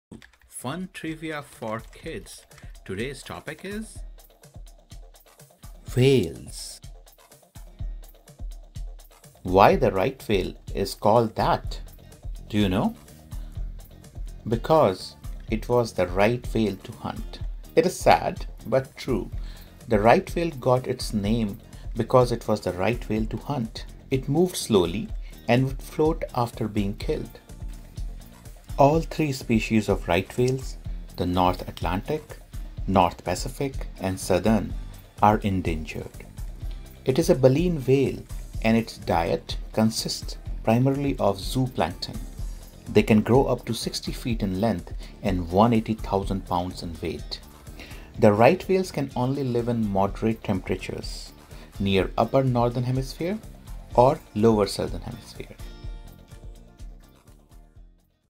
Why a Right whale is called that? | Fun Trivia for Kids | WS04
This is a shorts series -trivia for kids from different worlds - natural, fact, fiction. Today's topic is Whales.